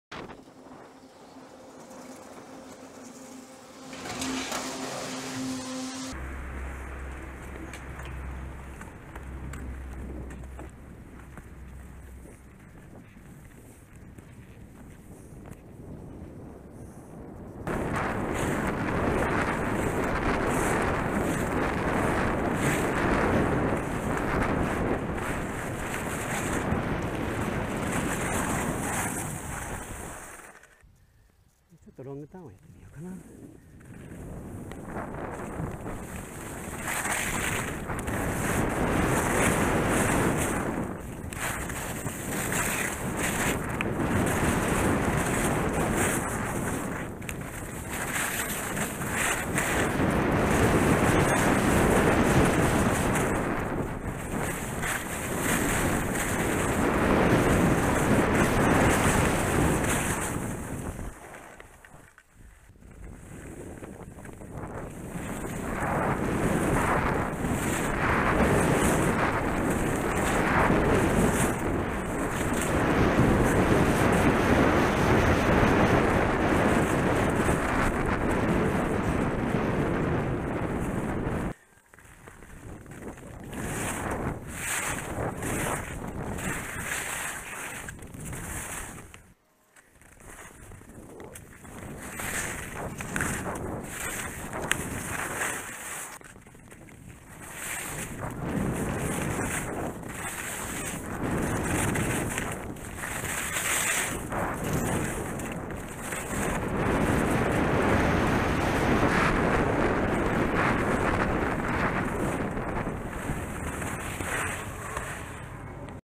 ニヤマ連休初日 フリコ沢から第1A線まで
函館ニヤマレジャーセンター・ニヤマ高原スキー場
フリコ沢から第1A線まで
2018/1/6